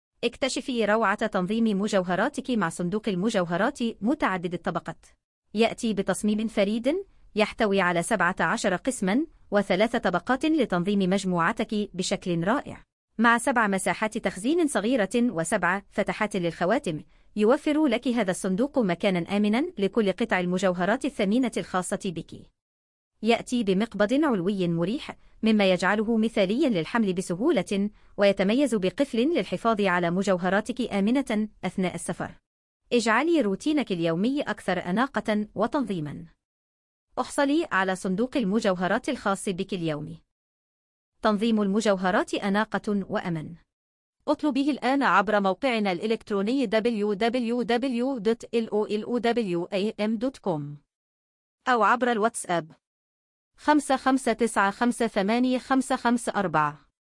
صندوق المجوهرات المميز للصبايا متوفر فوري في متجرنا في قطر
قناة تعرض لكم طريقة ترتيب التوزيعات بطريقة ملفتة جميلة بأفكار جديدة بإبداع جديد
صندوق مجوهرات راقي مكون من 17 قسمًا و3 طبقات، فيه 7 مساحات تخزين صغيرة، و7 فتحات للخواتم، ومرآة مدمجة، و6 خطافات قلادات على الجانبين. يمكن تخزين الخواتم والأقراط والقلائد والأساور والساعات
متين من الخارج وناعم من الداخل، مصنوع من جلد البولي يوريثين الصناعي، مبطن بالمخمل الزهري الناعم.
يحوي مقبض علوي مع قفل فهو مناسب للحمل بسهولة وللسفر حيث يمكن قفله.
يعتبر هدية رائعة وفاخرة للنساء والفتيات، بعد إغلاقه وطيه تصبح أبعاده 17 × 13 × 12 سم، فهو مناسب وسهل الحمل مما يجعله هدية مثالية، في مناسبات النساء مثل عيد الأم، والزواج والابنة وبنات الأخ أو الأخت وغيرهن وأعياد الميلاد.